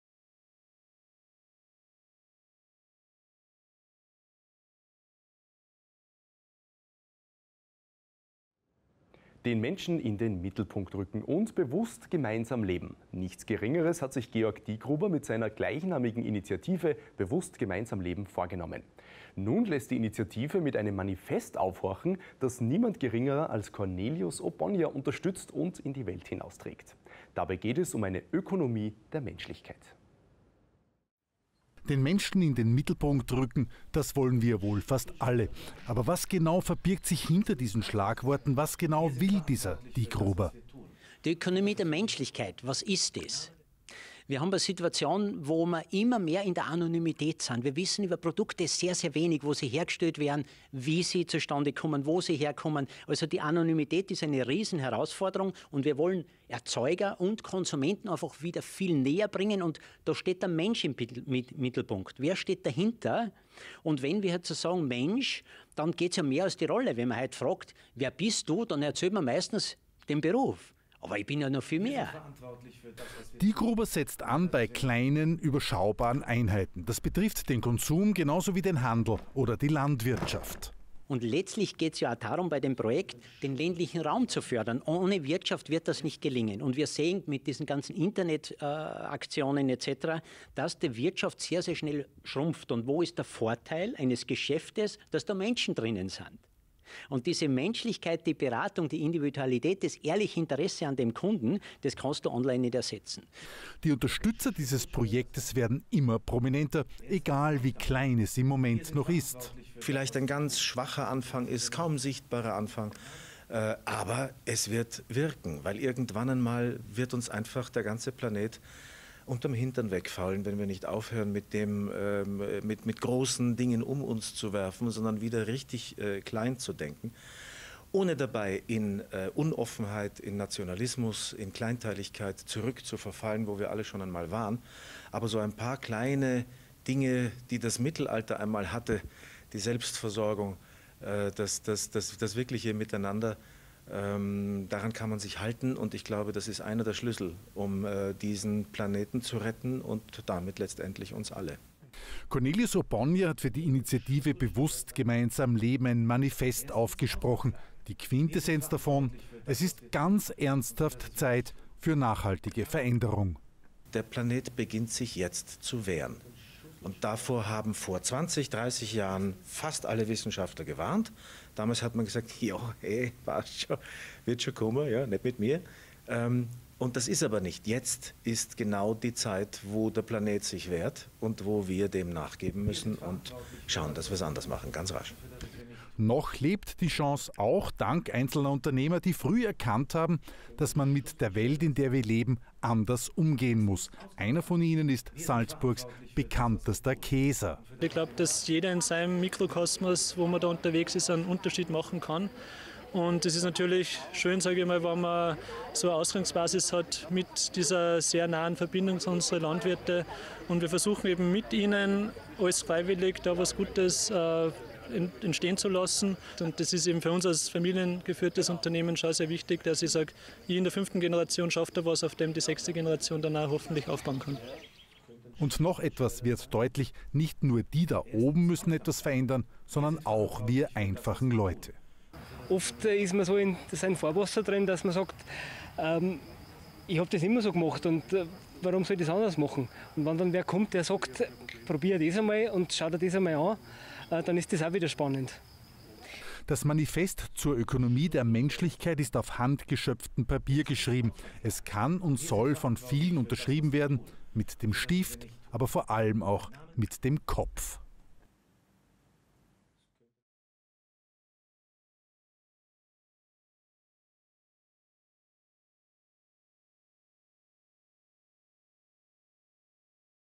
Cornelius Obonya fördert die Allianz Bewusst Gemeinsam Leben und eine Ökonomie der Menschlichkeit
Cornelius Obonya sagt Ja zu einer Ökonomie der Menschlichkeit, Ja zu einer Wirtschaft die dem Menschen dient.

Jeder Unternehmer muss Gewinn erzielen, aber Gewinn ist nicht alles. Ein Geschäft zu betreiben macht doch am meisten Spaß, wenn wir (und andere) Sinn darin sehen. Wir laden über das Manifest ein, eine Wirtschaft in Leichtigkeit, Freude und Vertrauen zu leben.
Ohne intakte Ökonomie verliert auch der gesunde Mensch seine Überlebensfähigkeit
(Dr. Günther Loewit, österr. Landarzt und Schriftsteller)